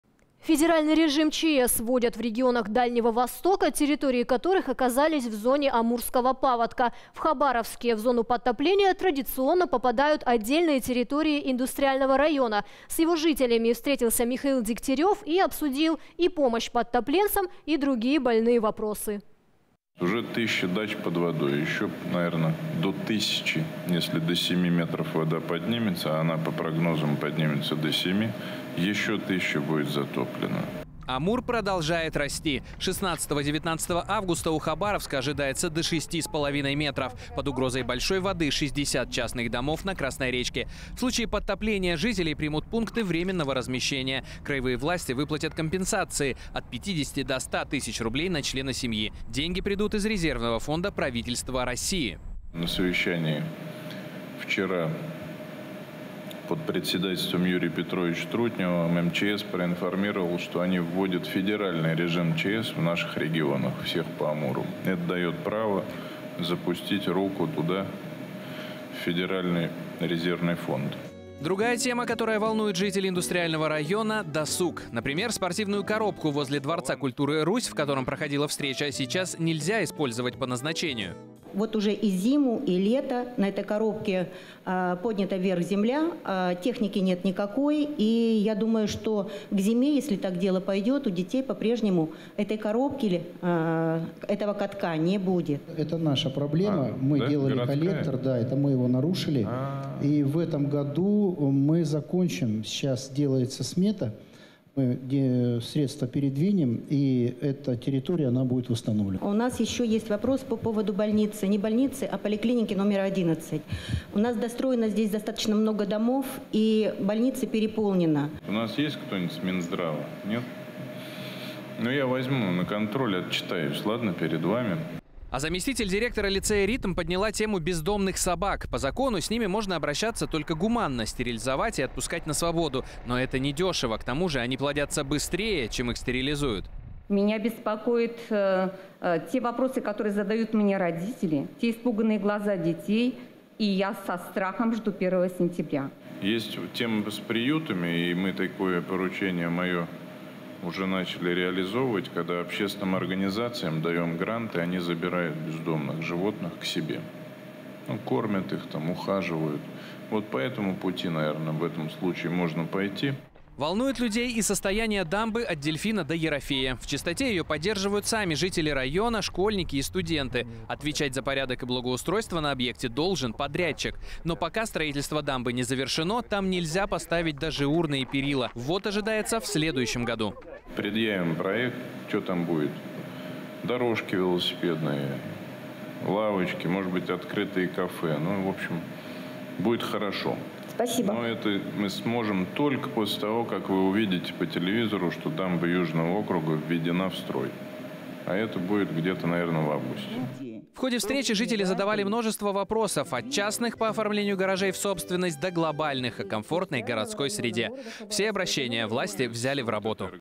Федеральный режим ЧС вводят в Хабаровском крае
Федеральный режим ЧС вводят в регионах Дальнего Востока, территории которых оказались в зоне амурского паводка. В Хабаровске в зону подтопления традиционно попадают отдельные территории Индустриального района — с его жителями встретился Михаил Дегтярёв, на которой обсудил и помощь подтопленцам, и другие больные вопросы.
Амур продолжает расти — 16-19 августа у Хабаровска ожидается до 6,5 метров. Под угрозой большой воды находятся 60 частных домов на Красной речке. В случае подтопления жителей примут пункты временного размещения, краевые власти выплатят компенсации — от 50 000 до 100 000 рублей на члена семьи. Деньги придут из резервного фонда правительства России.